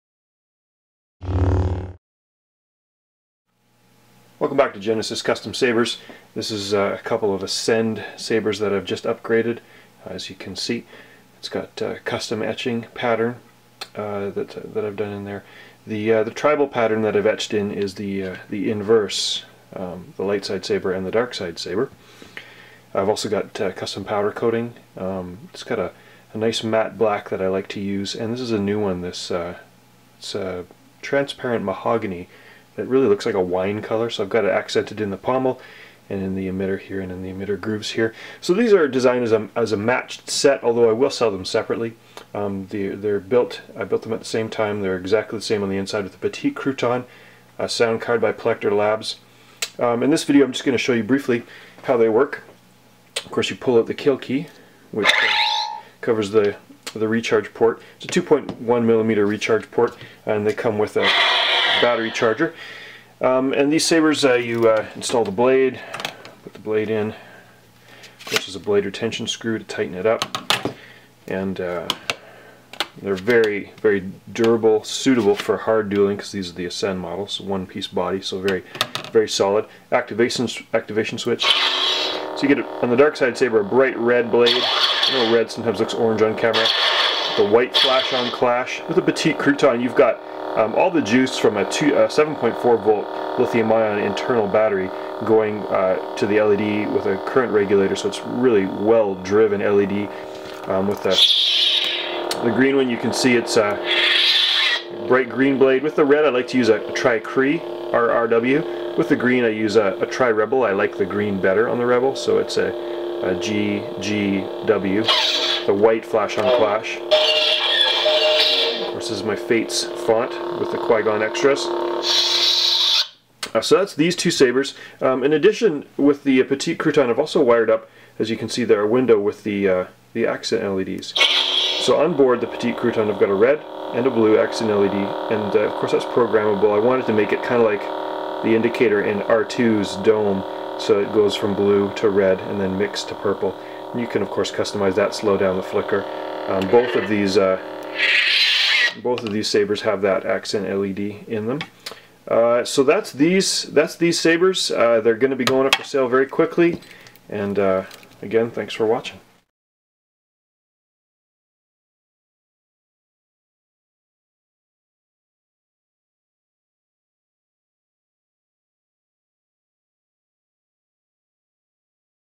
Etched Pair
An elegant pair of Ascend sabers with custom acid etching and Petite Crouton sound cards.